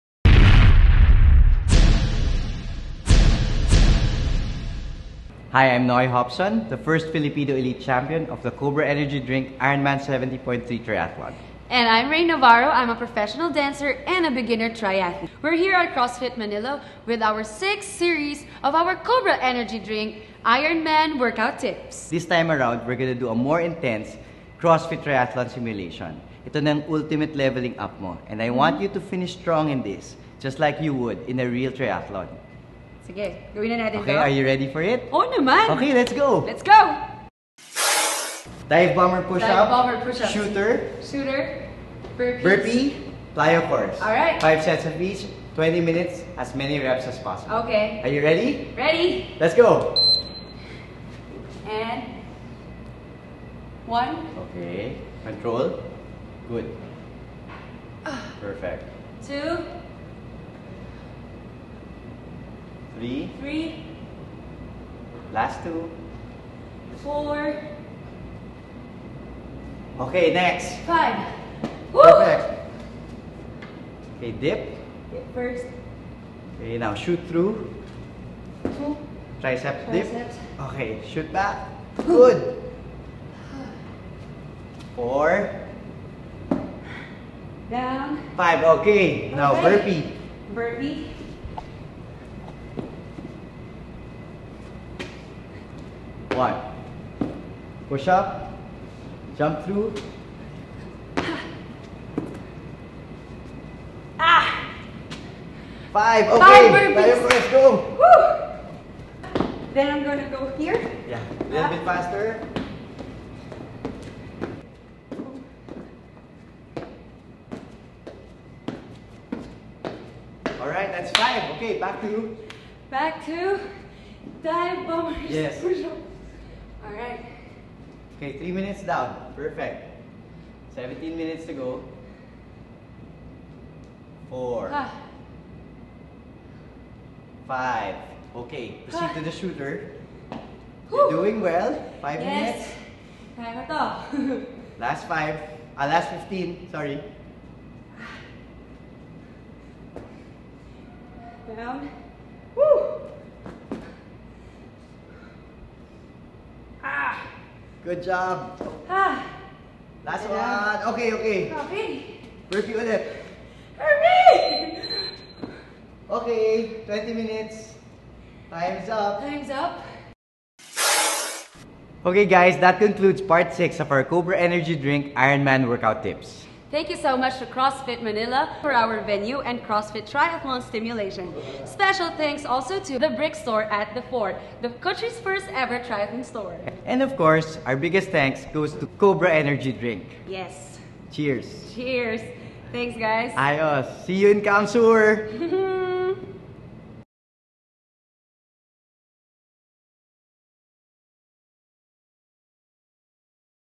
Cobra Energy Drink IronMan Workout tips with Coach Noy Jopson (Part 5, Level Up Triathlon)
A series of workout videos entitled, Cobra Energy Drink Ironman Workout Tips of the Day where first Filipino Elite Champion of the Cobra Ironman 70.3 Triathlon, Coach Noy Jopson coaches professional dancer, Rein Navarro with workouts that are meant to improve one's swim muscles, bike muscles and running muscles. It's interesting to watch because you'll see how Rein starts out as just a triathlete wannabe and grows into a beginner triathlete as the workouts progress. The workout tips videos are functional and fun; meant for serious athletes and anyone and everyone who wants to stay fit.